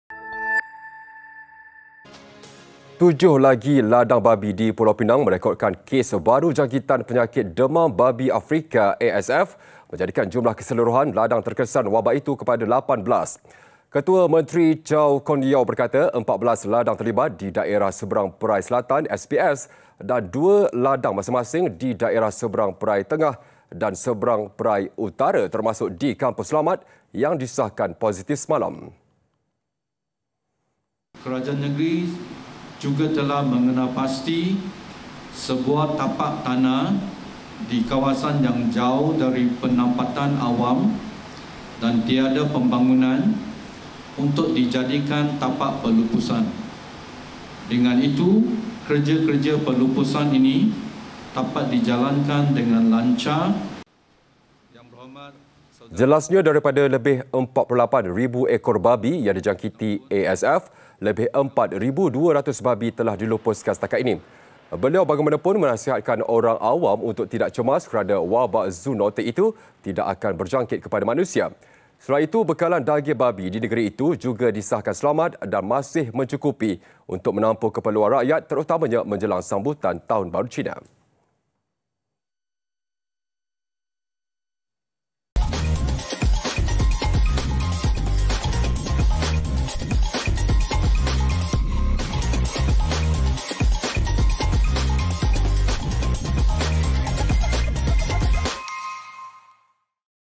DEMAM BABI AFRIKA | 18 Ladang Babi Telah Dijangkiti
Tujuh lagi ladang babi di Pulau Pinang merekodkan kes baharu jangkitan penyakit Demam Babi Afrika (ASF) menjadikan jumlah keseluruhan ladang terkesan wabak itu kepada 18.

Ketua Menteri, Chow Kon Yeow, berkata 14 ladang terlibat di daerah Seberang Perai Selatan (SPS) dan dua ladang masing-masing di daerah Seberang Perai Tengah dan Seberang Perai Utara, termasuk di Kampung Selamat yang disahkan positif semalam.